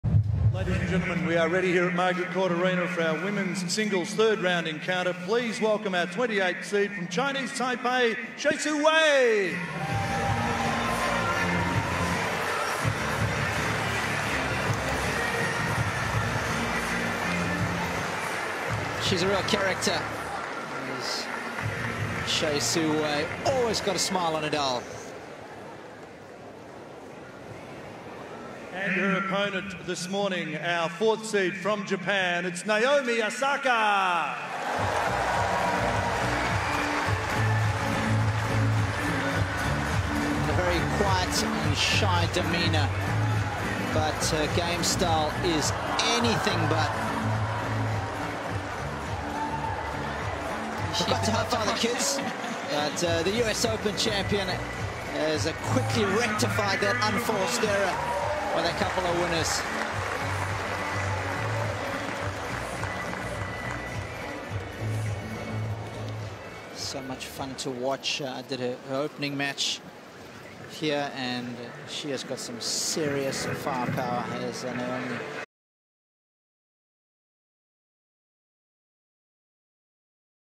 Naomi Osaka & Su-Wei Hsieh enter Margaret Court Arena | Australian Open 2019
Naomi Osaka & Su-Wei Hsieh enter Margaret Court Arena for their third round match.